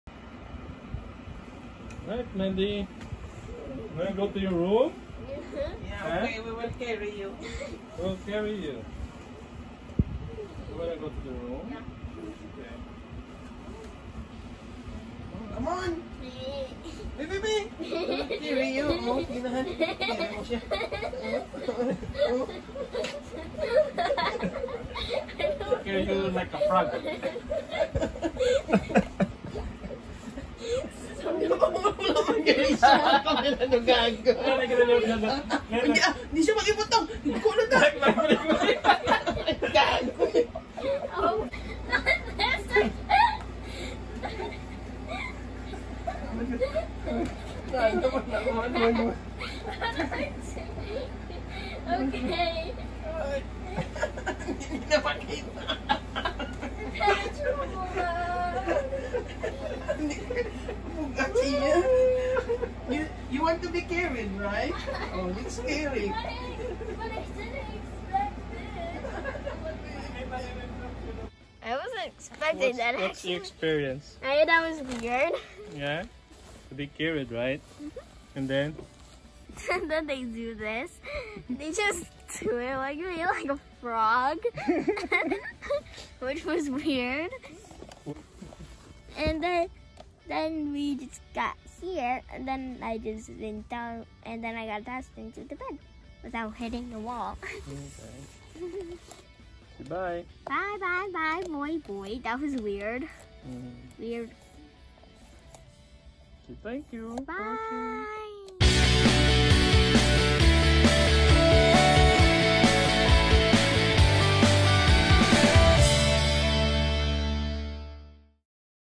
MOM and DAD PRANK DAUGHTER on Bed
Parent prank daughter! Mom and dad want to sleep but daughter don’t want to transfer to her room unless she will be carried by dad. But Instead Mom and dad carried their daughter to her room. Just for fun! 🤩🤣😂.